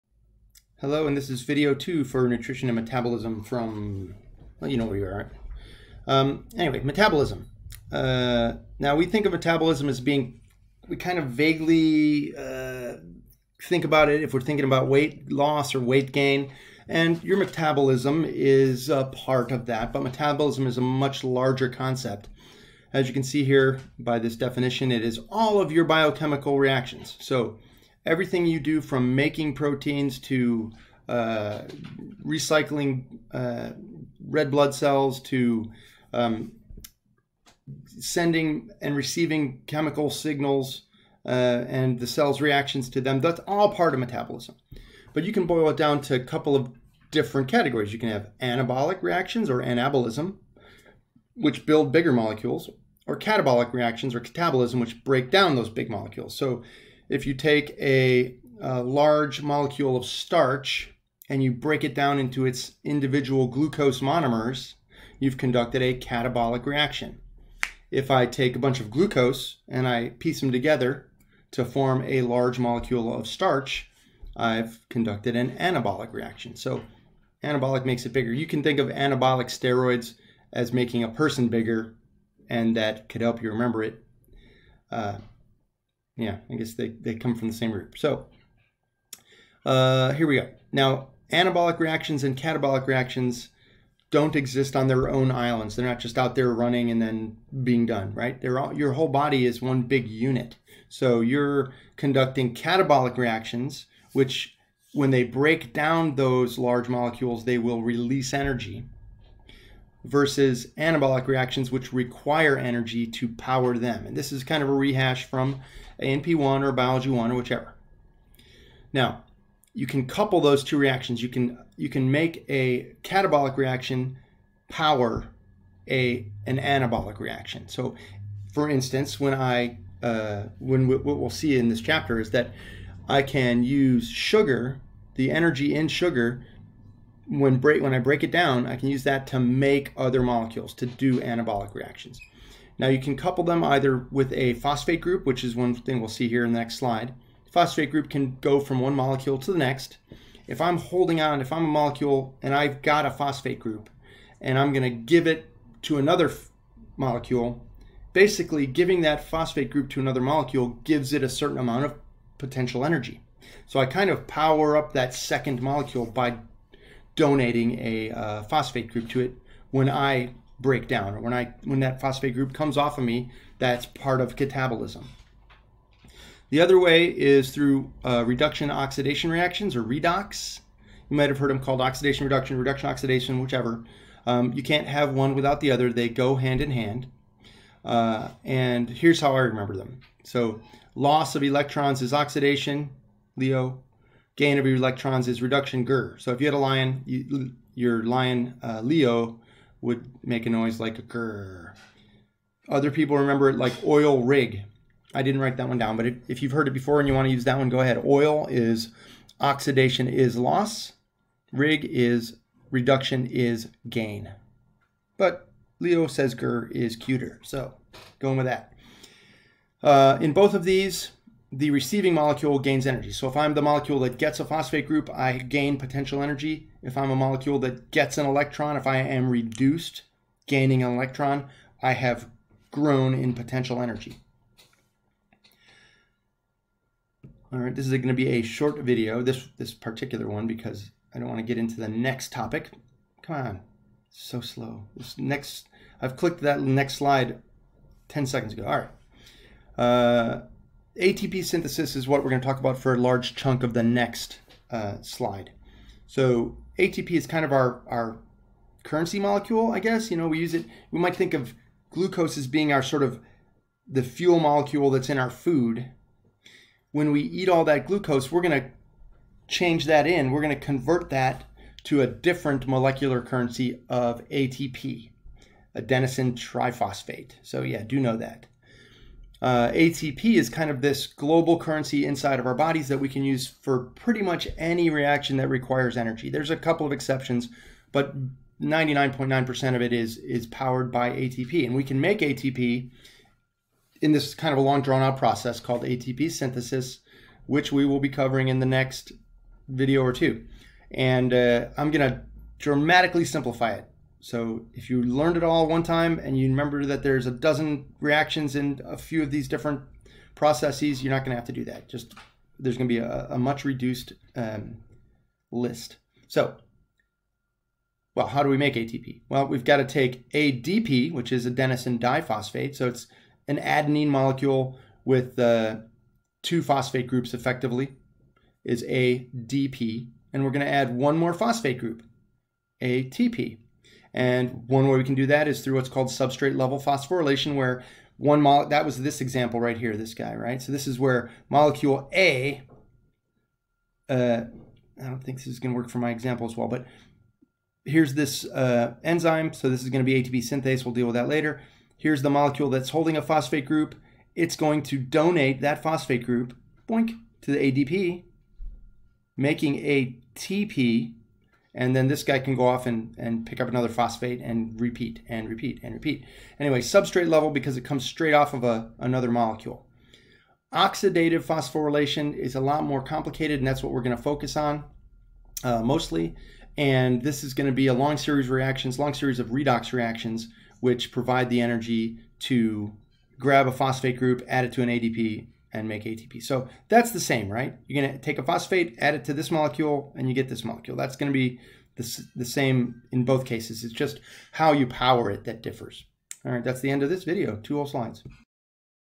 2402 Lecture Chapter 24 Nutrition and Metabolism 2 PowerPoint Screencast
Intro to Metabolism